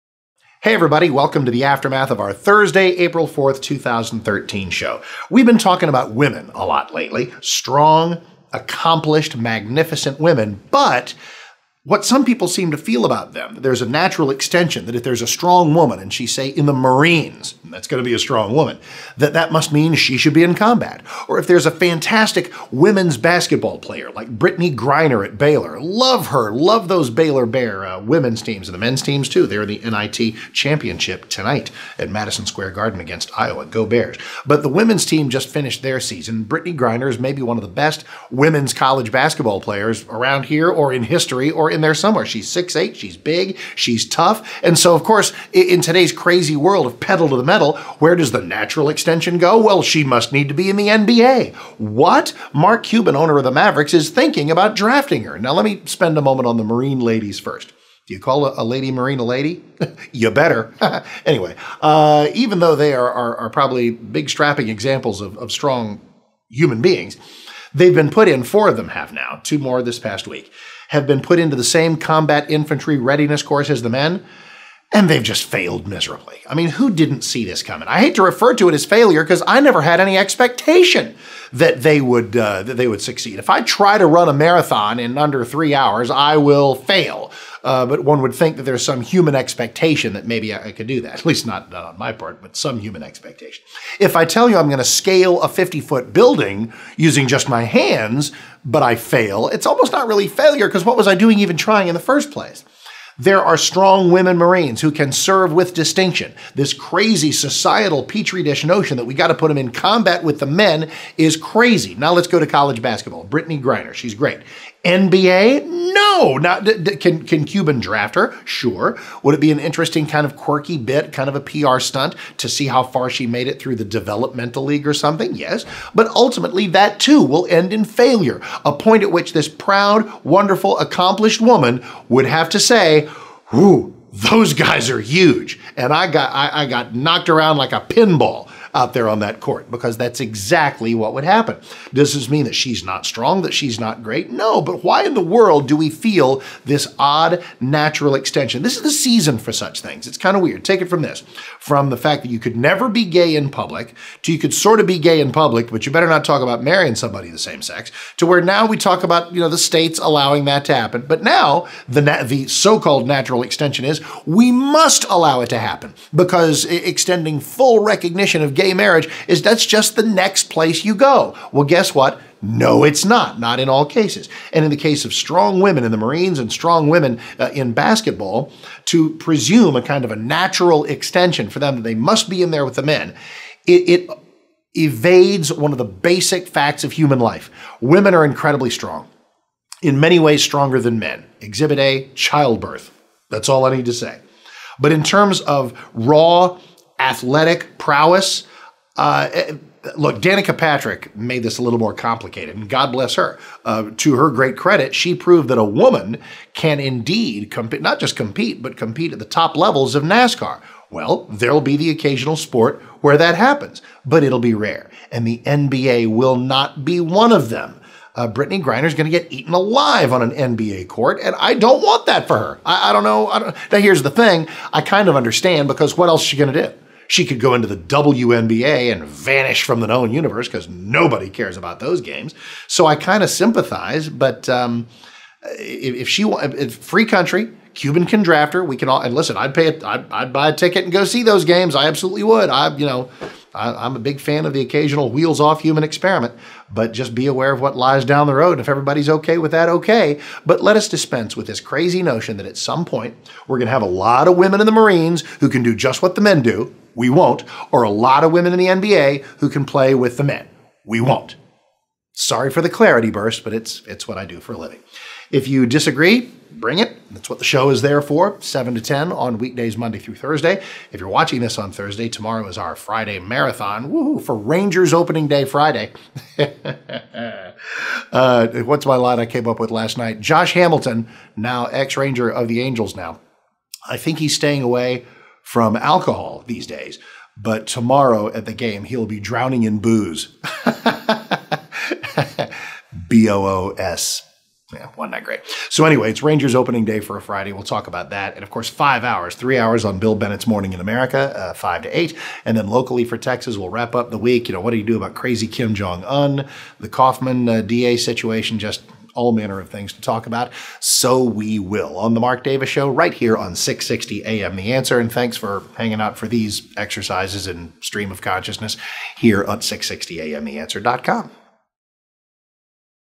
Women, combat and the NBA — Mark Davis video blog; 4.4.13
Mark Davis video blog; 4.4.13